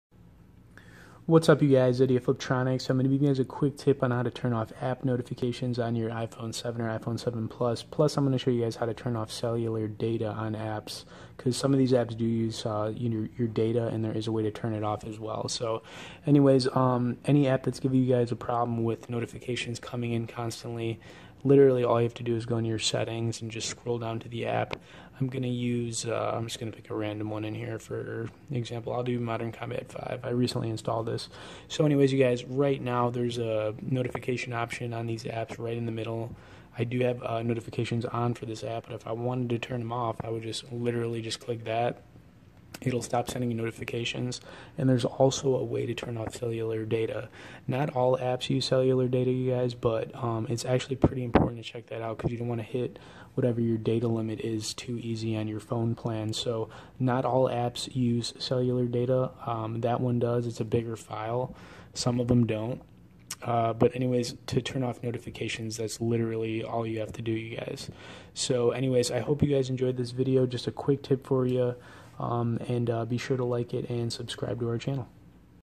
Iphone 7 / Iphone 7 Plus - How To Turn Off App Notifications - Fliptroniks.com
For how to turn off app notifications on iphone 7 / iphone 7 plus your going to first want to go into your settings. In this example I used modern combat 5 which I simply clicked on the app and then turned off its notifications.